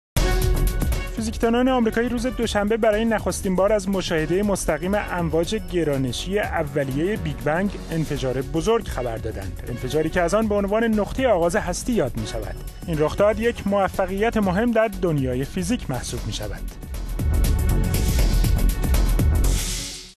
مشاهده مستقیم امواج گرانشی بیگ بنگ
*خبر کوتاه*

فیزیکدانان آمریکایی روز دوشنبه برای نخستین بار از مشاهده مستقیم امواج گرانشی اولیه بیگ بنگ، انفجار بزرگ خبر دادند. انفجاری که از آن به عنوان نقطه آغاز هستی یاد می شود.

این رخداد یک موفقیت مهم در دنیای فیزیک محسوب می شود.

وجود امواج ناشی از بیگ بنگ در نظریه نسبیت آلبرت انیشتین پیش بینی شده است.